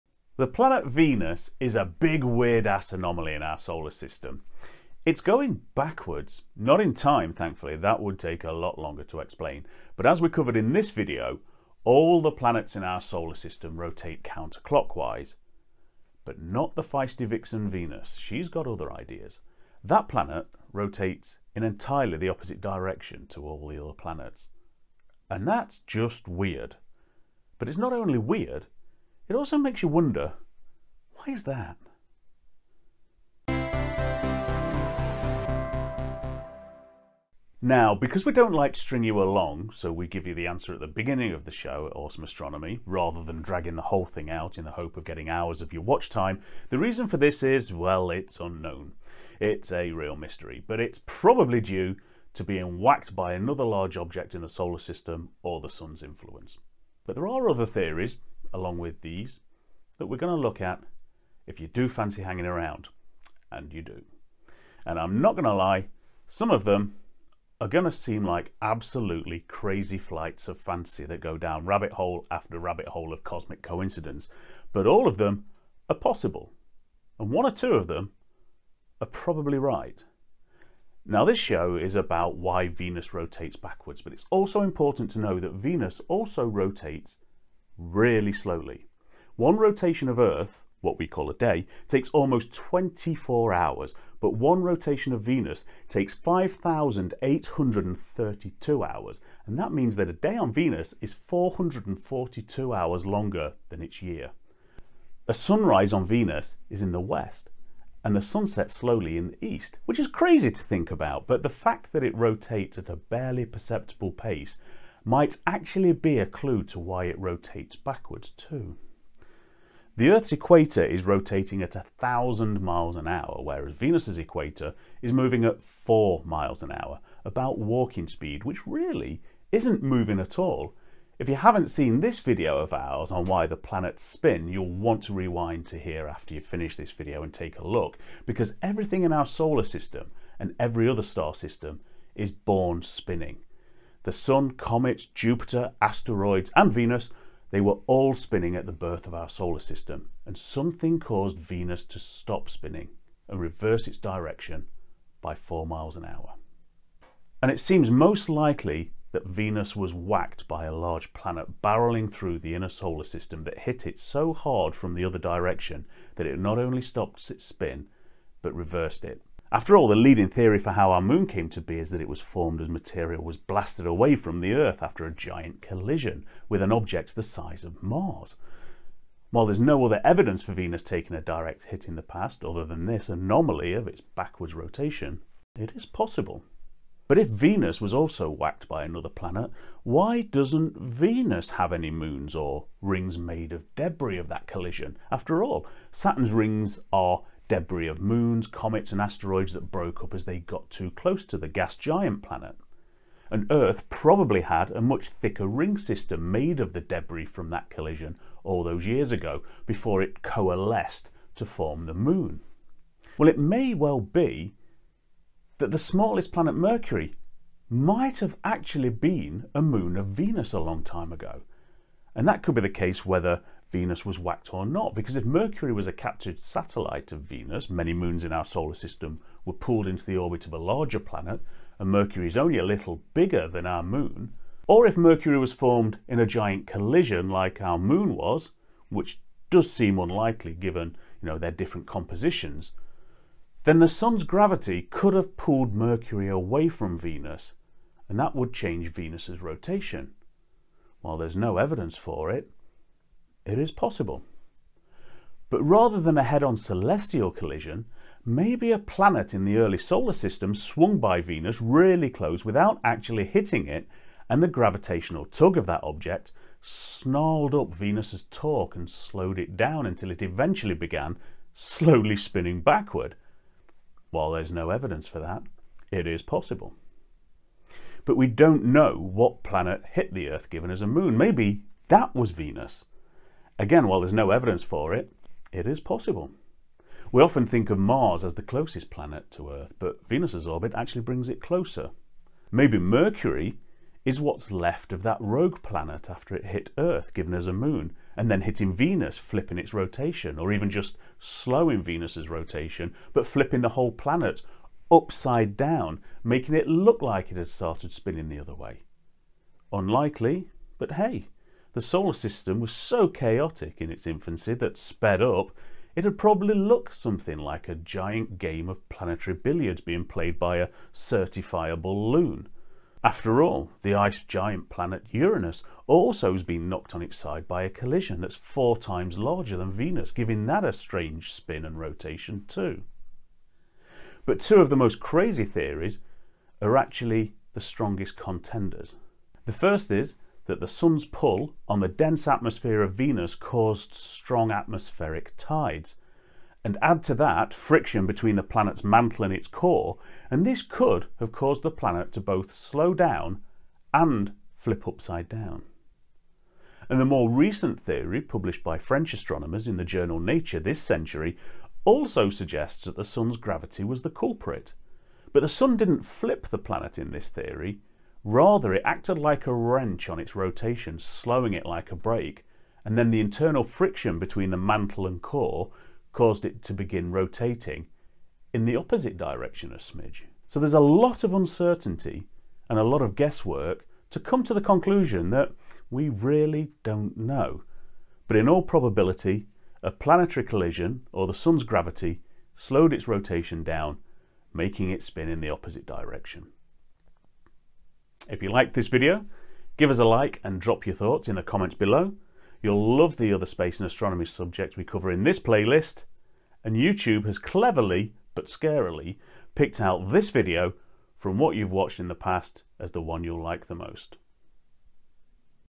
Venus is going backwards
All the planets in the solar system rotate counter clockwise - except Venus.

A day on Venus lasts longer than it’s year!

This makes Venus a bit of a space oddity, so this video looks at what’s going on and a few theories explaining why.

And if you want to hear more from us we have 2 podcast episodes each month: